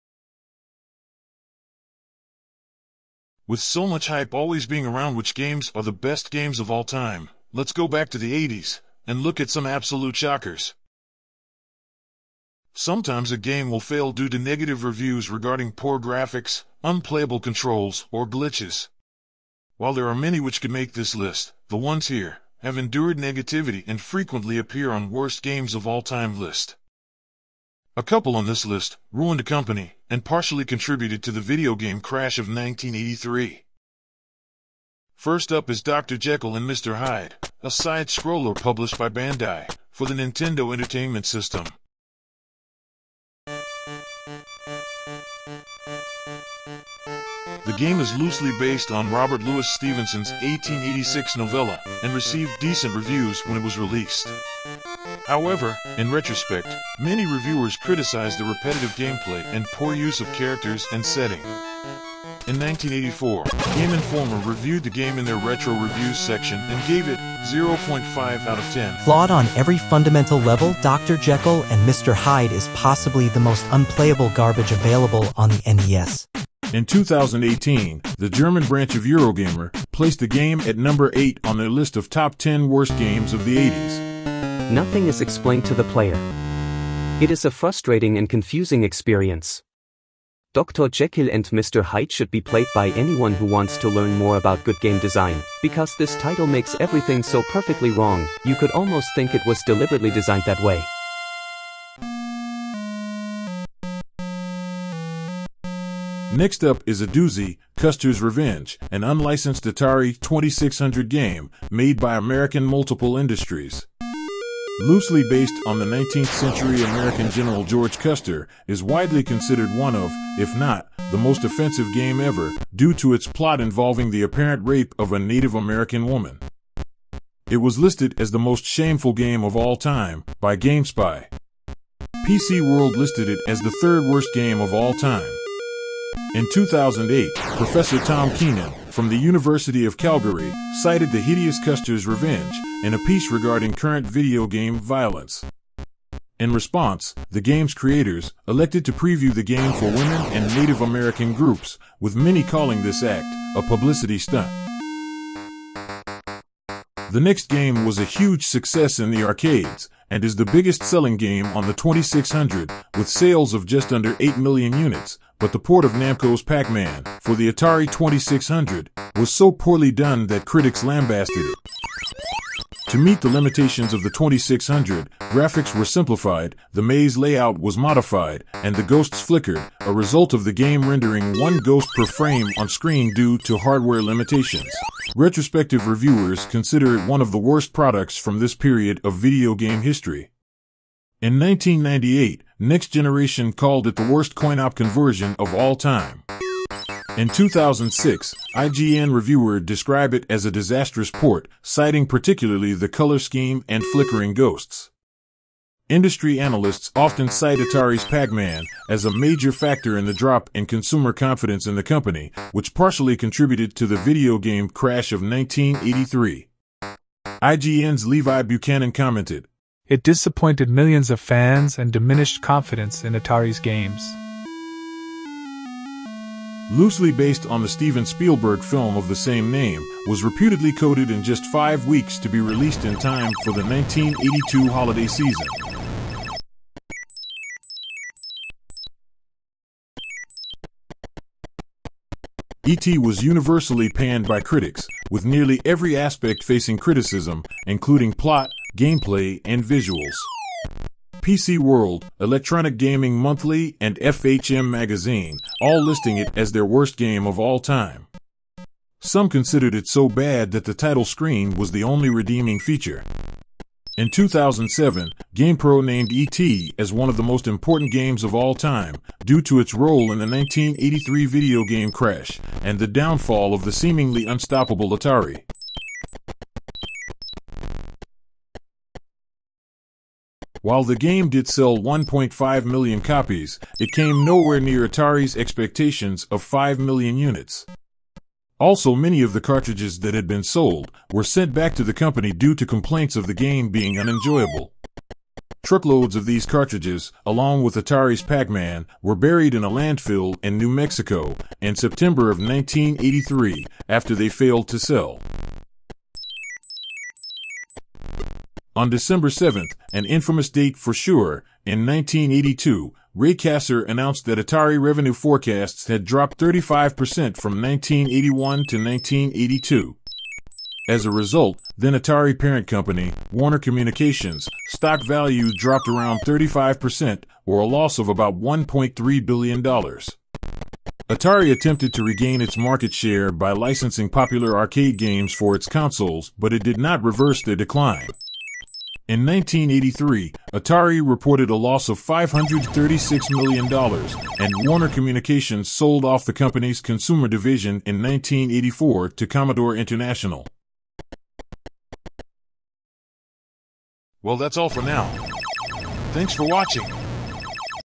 Worst Games Ever! The 1980s (I'm looking at you, Atari!)
A retrospective look back at some of the all-time worst video games in history. This list is far from extensive and could include many tie-in games for TV shows or movies which are generally considered "shovelware".